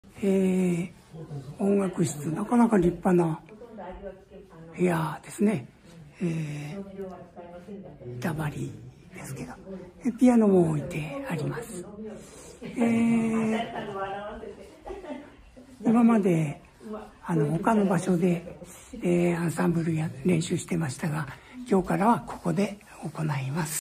ヨシオのハモニカライフ№16
今日は、毎年合同発表会を開いているアミカスに来ています。これまでは別の会場で行っていたアンサンブルの練習を、このアミカス音楽室で行うことにしました。